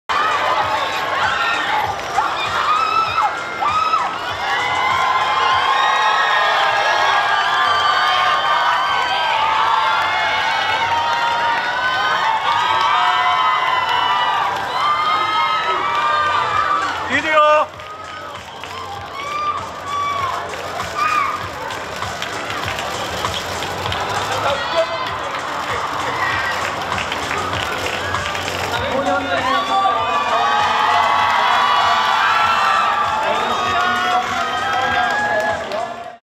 배우 송강호 제 27회 부산국제영화제 레드카펫 [2022 BIFF] Song Kang-ho red carpet
배우 송강호가 5일 오후 부산 해운대구 영화의 전당에서 열린 제 27회 부산국제영화제 개막식에 참석해 레드카펫을 밟고 있다.